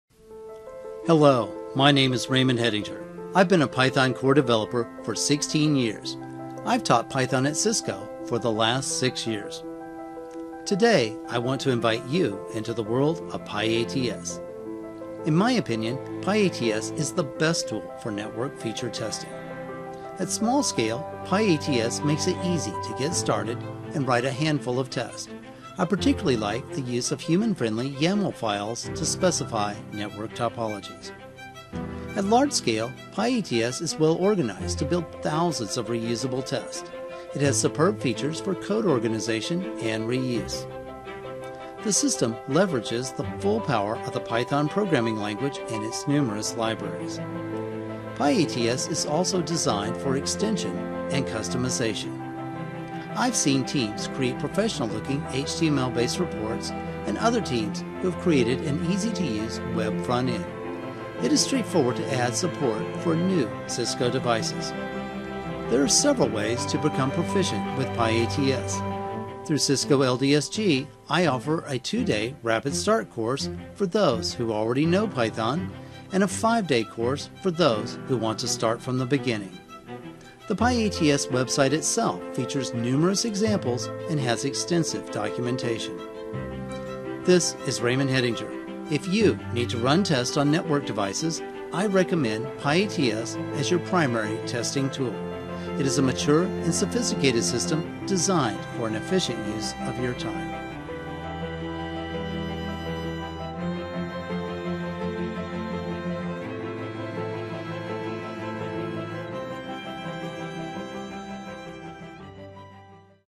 pyATS for your Test Automation
Extensible by design, pyATS enables developers to start with small, simple and linear test cases and easily scale towards large, complex and asynchronous test suites. Raymond Hettinger is an veteran Python core developer currently teaching Python and pyATS in Cisco.